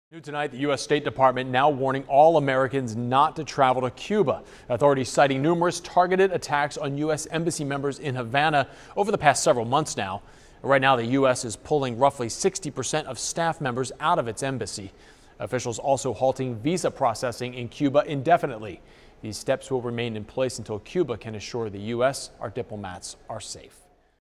US to warn American travelers not to visit Cuba
The United States is warning Americans against visiting Cuba and ordering more than half of U.S. personnel to leave the island, senior officials said Friday, in a dramatic response to what they described as “specific attacks” on diplomats.